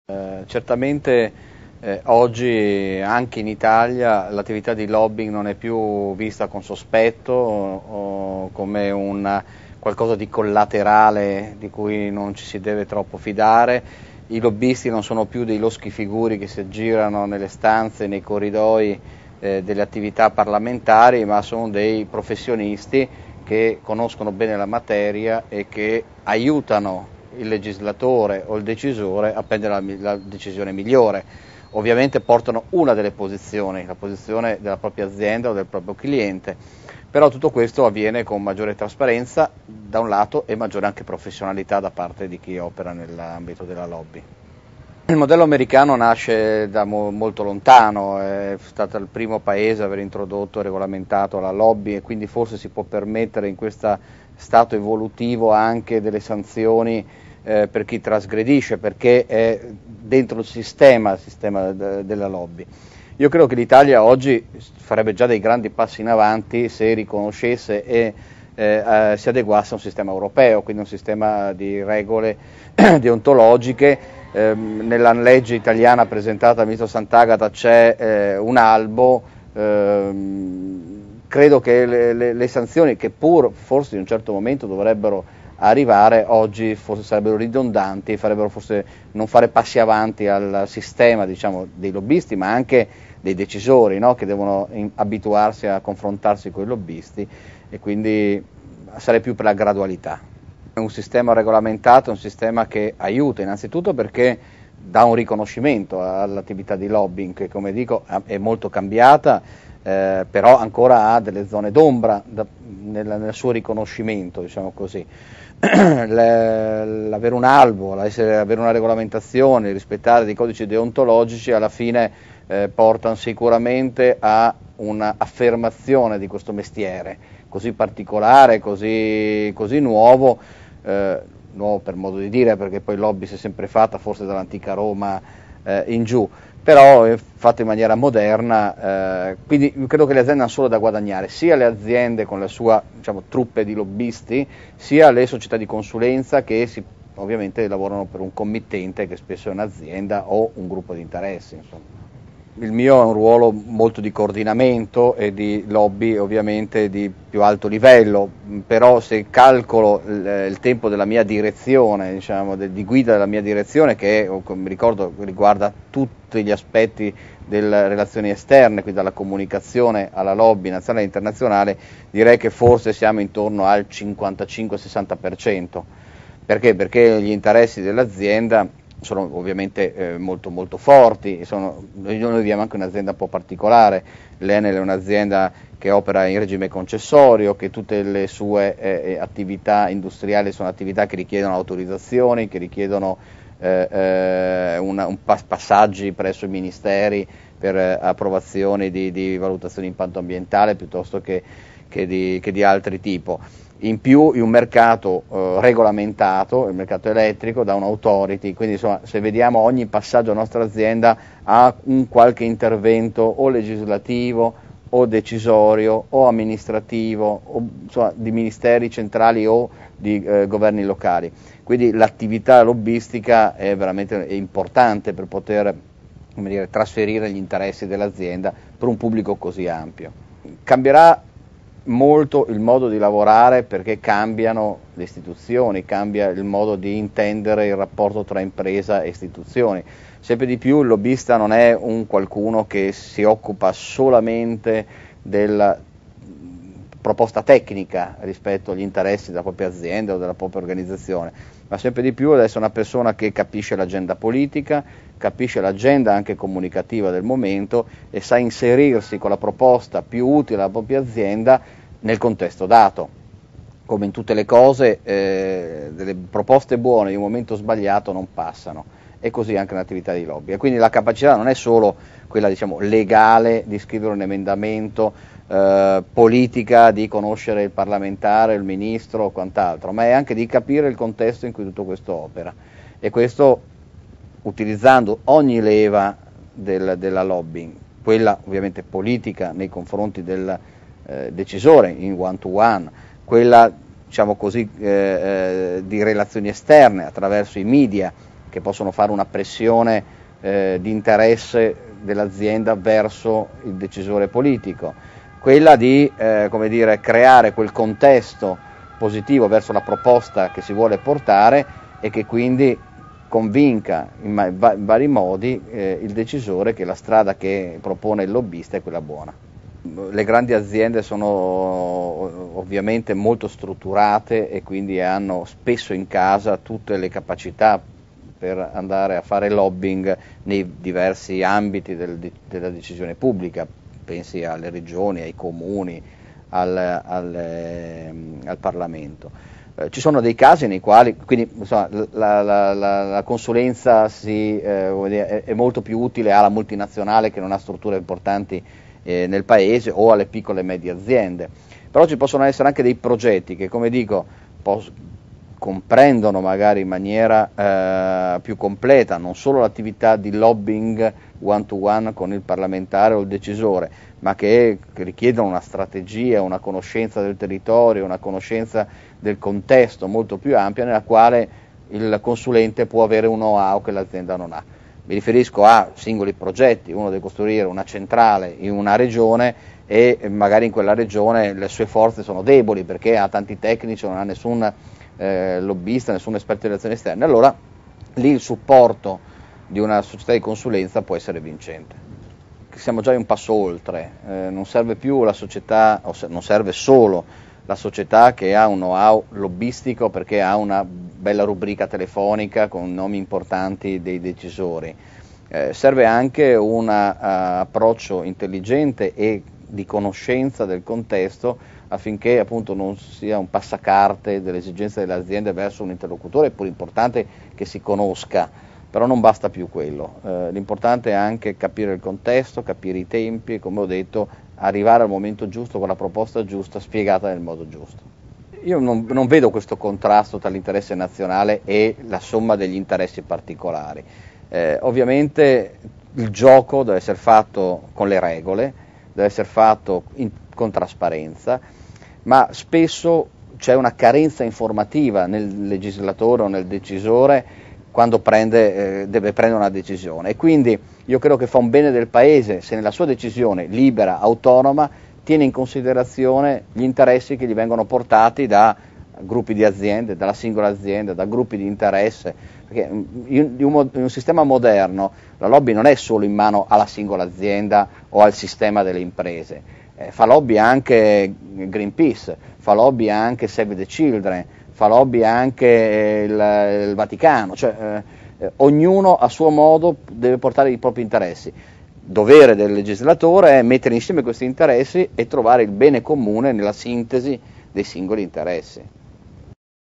Comin-Anche in Italia i lobbisti non sono più losche figure
Il direttore delle relazioni esterne di Enel a SherpaTv: «Le sanzioni oggi sarebbero ridondanti e non farebbero fare passi in avanti al sistema»